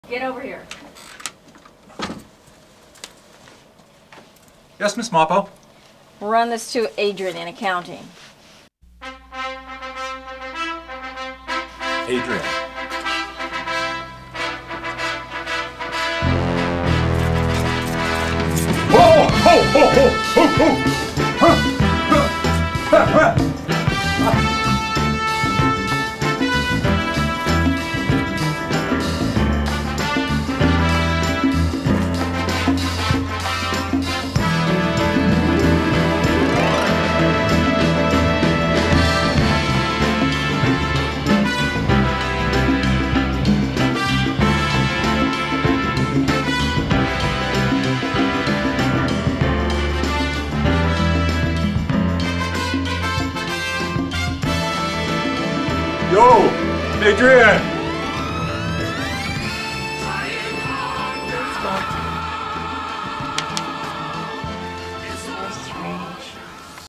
Yo Adrian
Bob turns into his favorite alter-ego Rocky to deliver the mail.